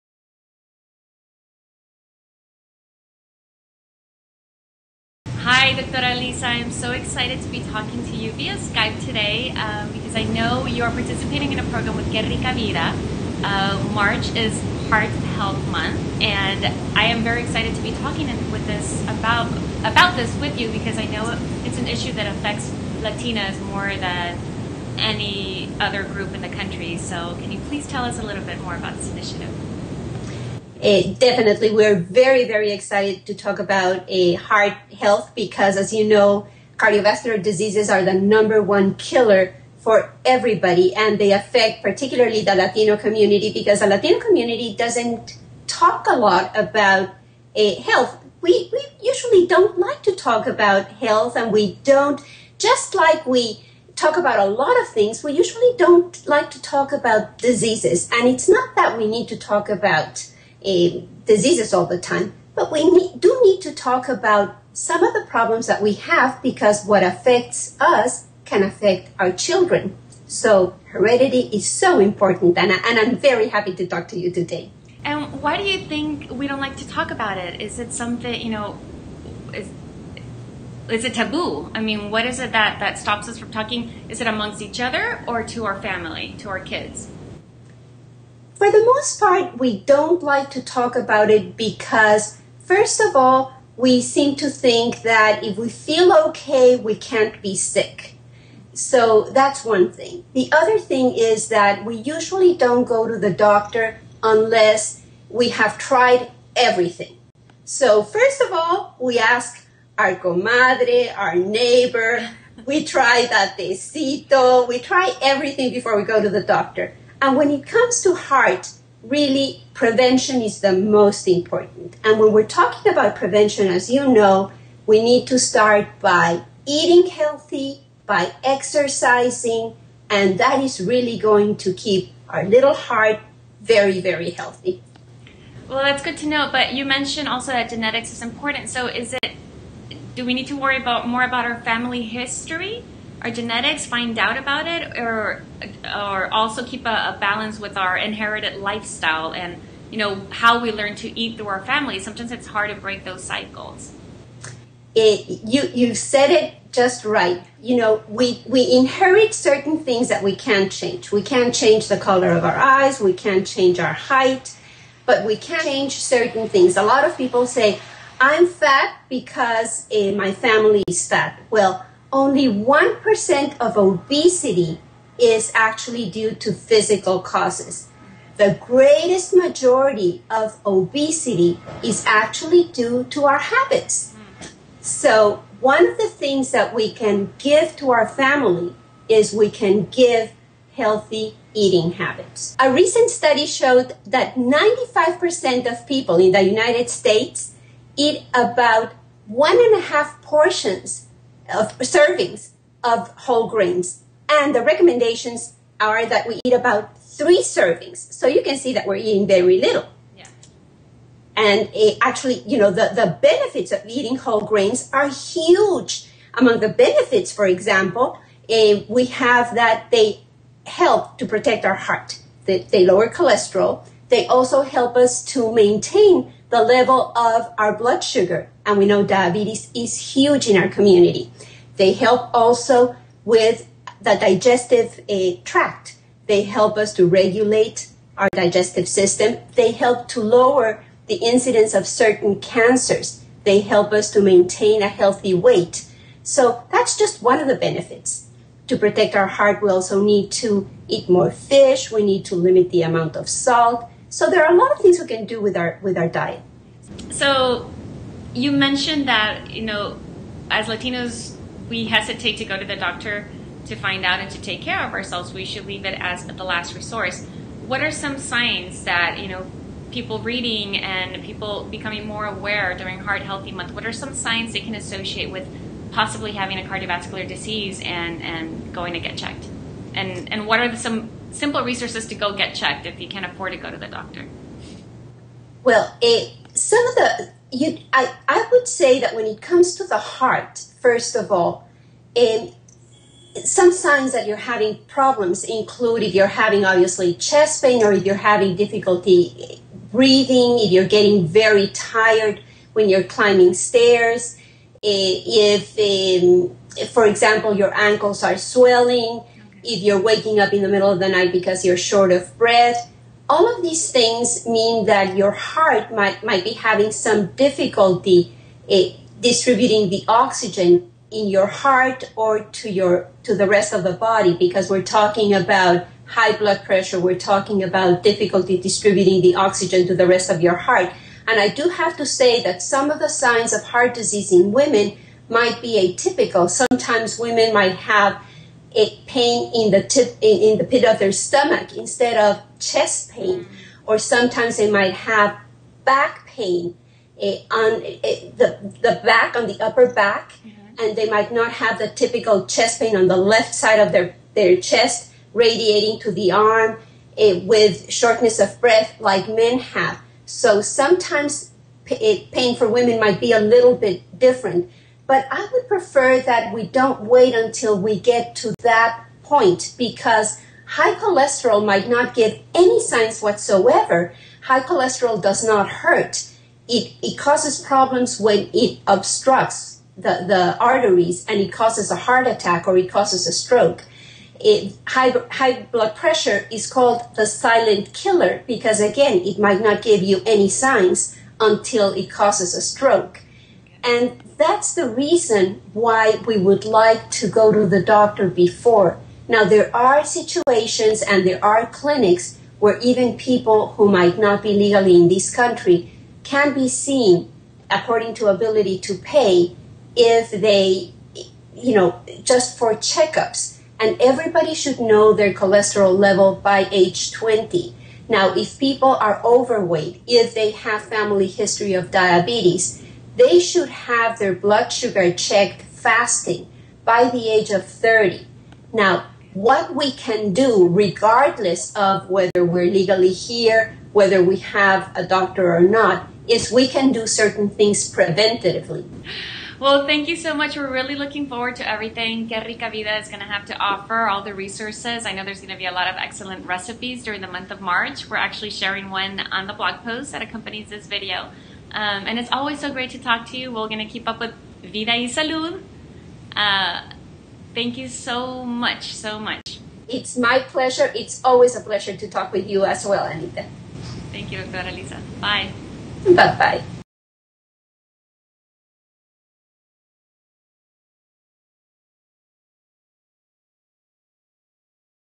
Interview with Dra. Aliza Lifshitz | SpanglishBaby
talk to Dra. Aliza Lifshitz about how heart disease affects Hispanic families and tips to prevent it and recognize its symptoms.